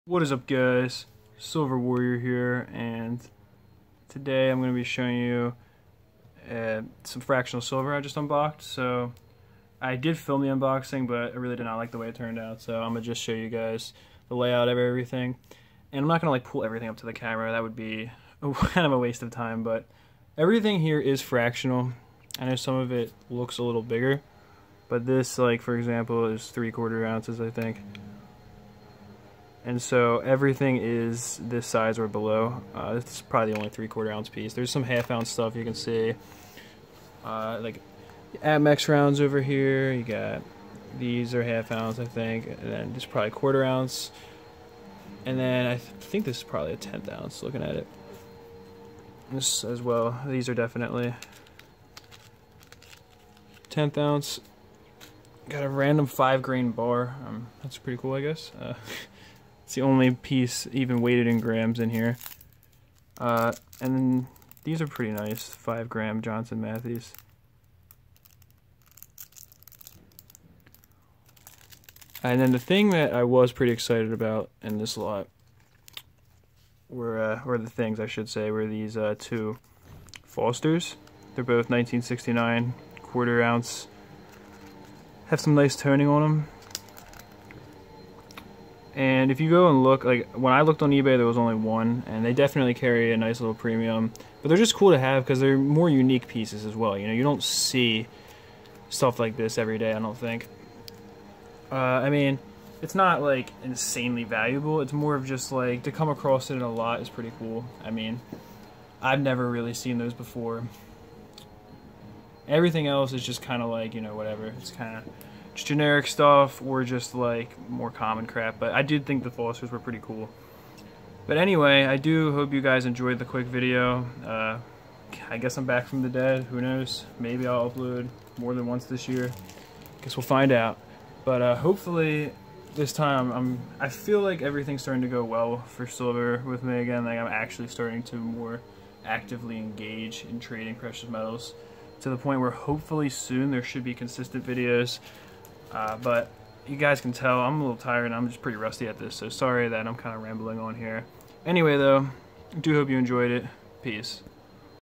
Variety Fractional Silver Pickup
Sorry about the mistake lol, the small stack of fractionals consisted of 3 1/10th ounce pieces and 4 1 gram pieces, not all 1/10th